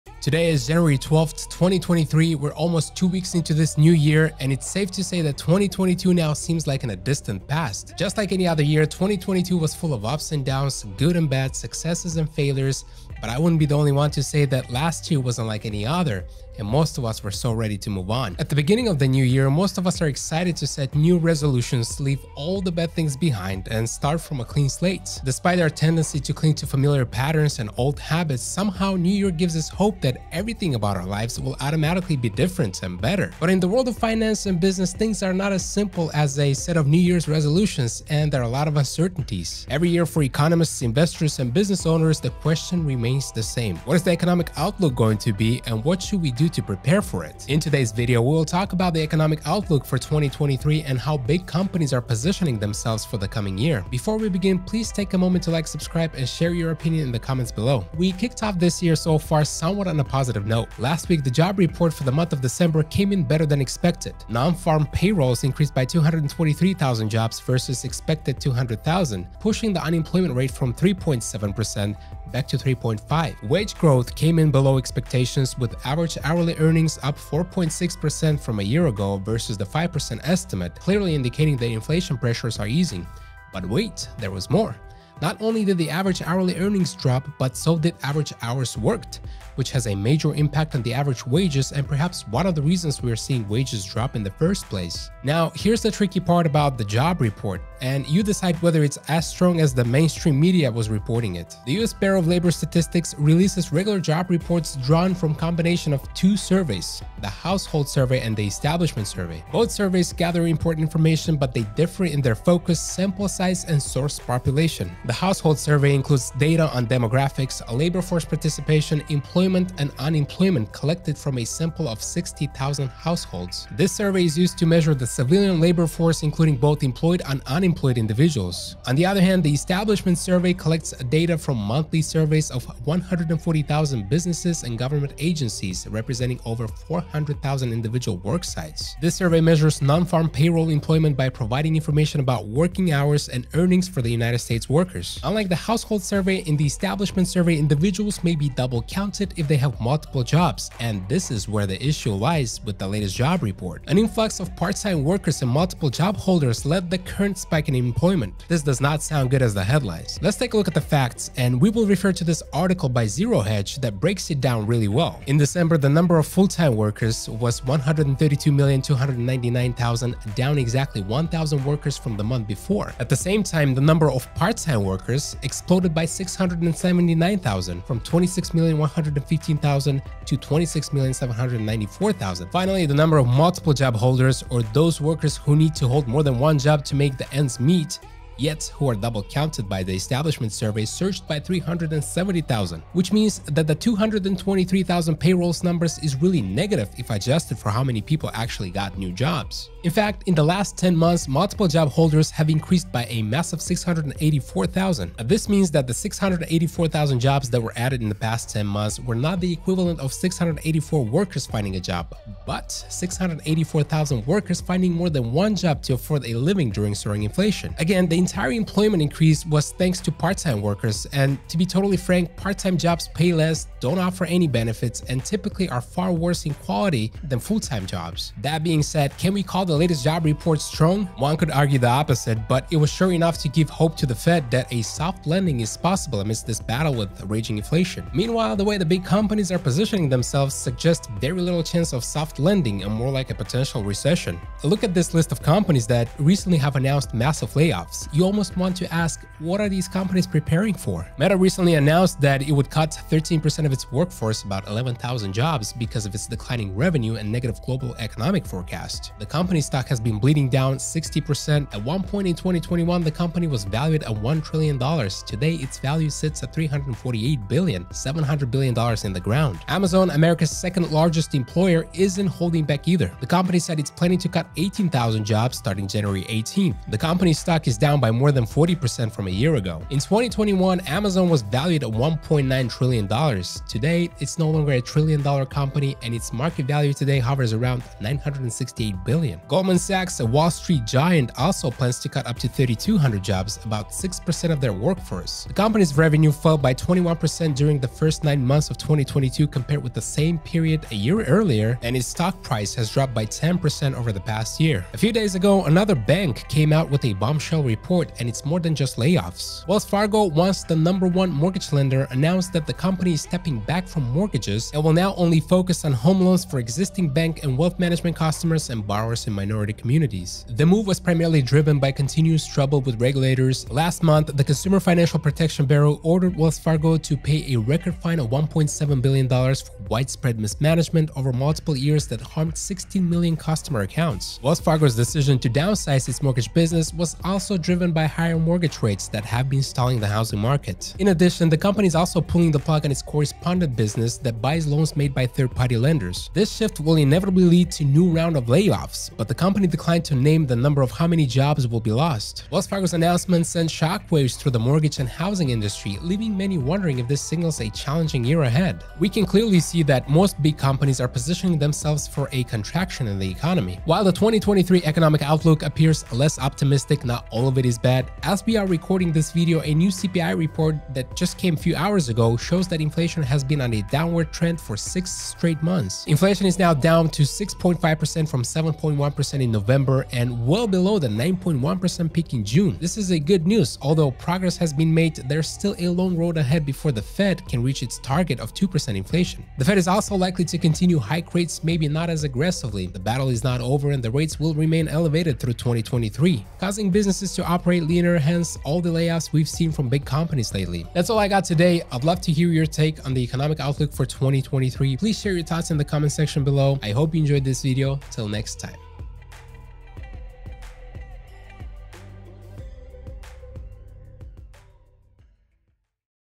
2023 Layoff Bloodbath
The latest job report came in strong, but is it as strong as the mainstream media reported it? In this video we dig deeper into the job report, and also we talk about the economic outlook for 2023, and how big companies are positioning themselves for the year ahead. Oh, and did I mention layoffs?